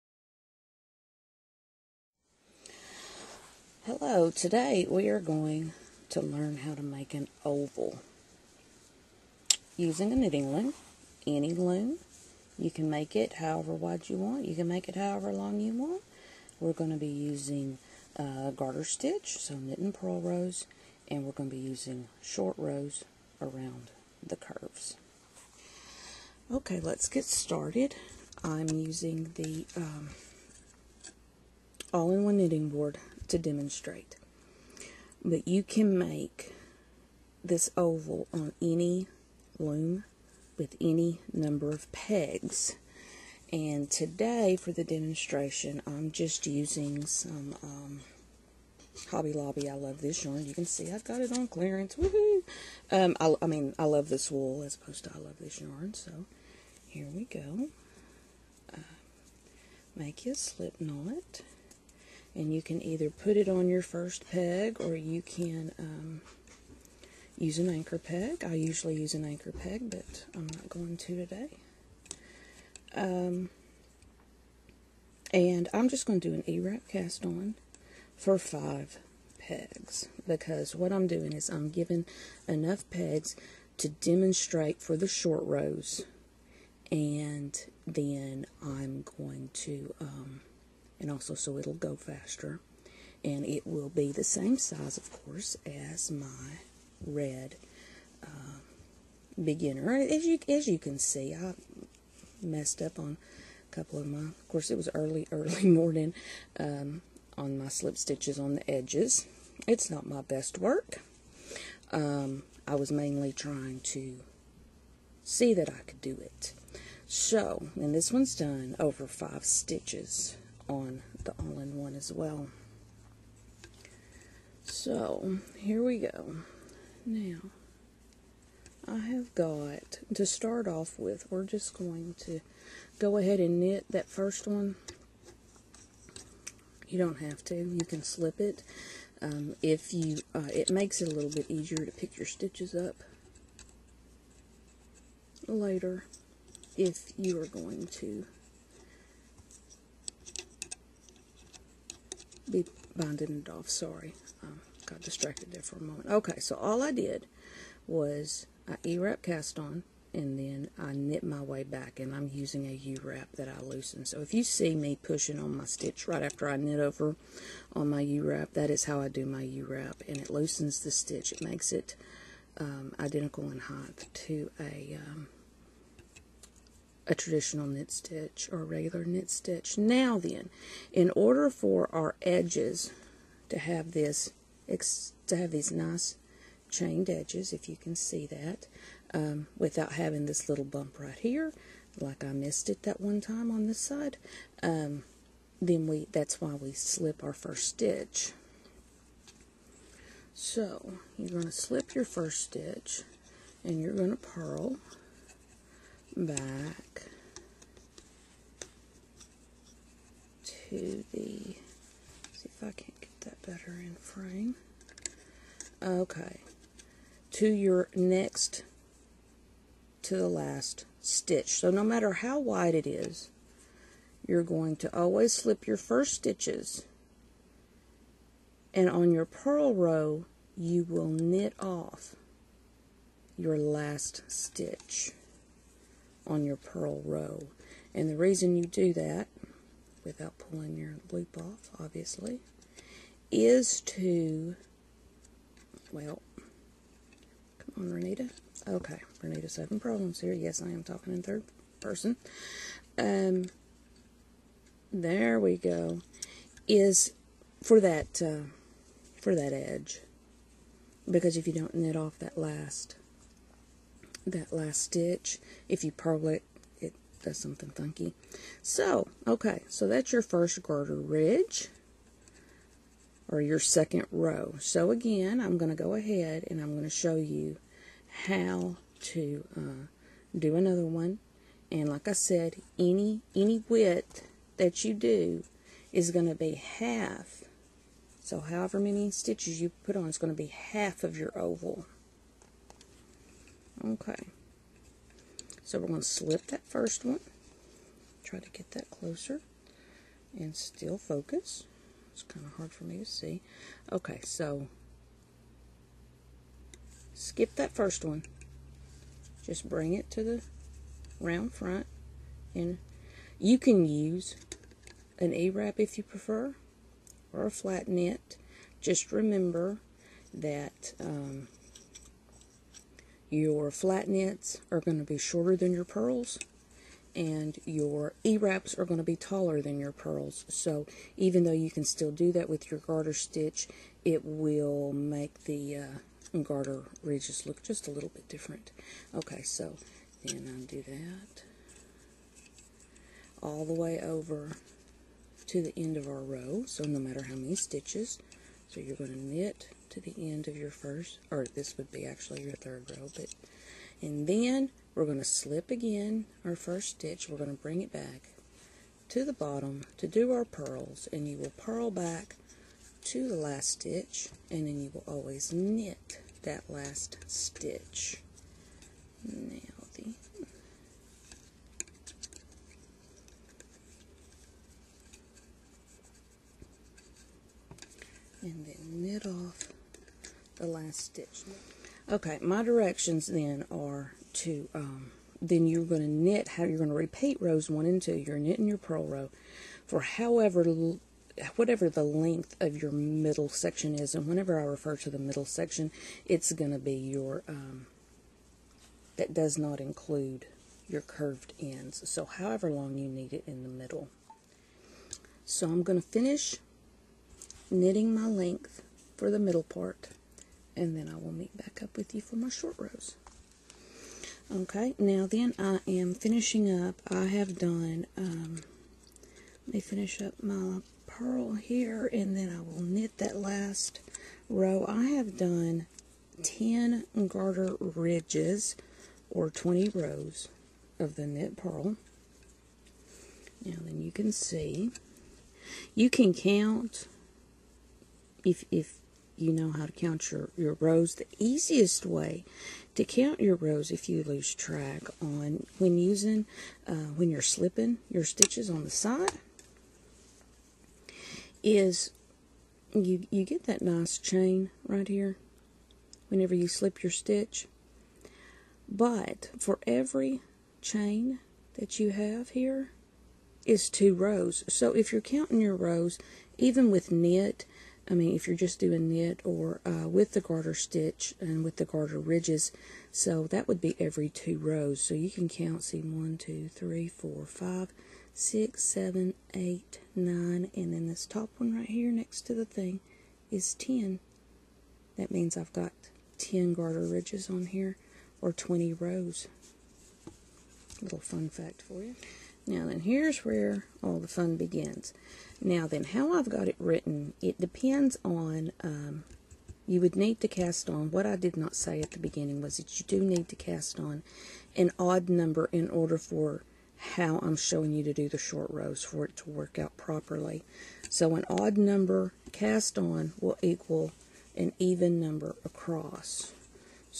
Loom Knit Oval - Part 1
This is part 1 of 5 of the tutorial on how to make an oval on a knitting loom.  Any loom can be used.  You can make any size oval with this technique.

Part 1 includes the introduction and knitting the 1st side of the middle section.

You can find the written instructions and other free patterns on my blog at stringtheoryfiberworks.blogspot.com .